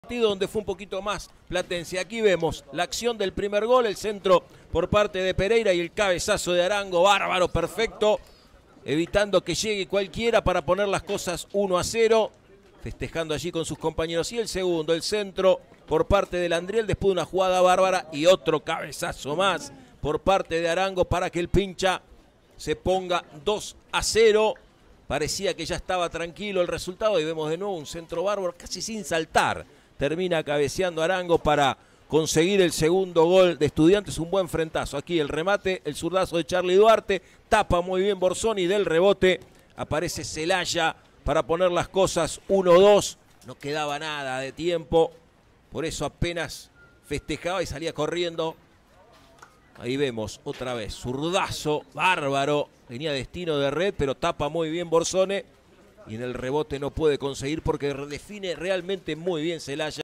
ESTUDIANTES 2 - 1 PLATENSE I Resumen del partido I CopaProyección Sur Finanzas 2024
Partidazo en La Plata, con triunfo del Pincha sobre el Calamar, por la fecha4 de la CopaProyección Sur Finanzas 🏆 2024.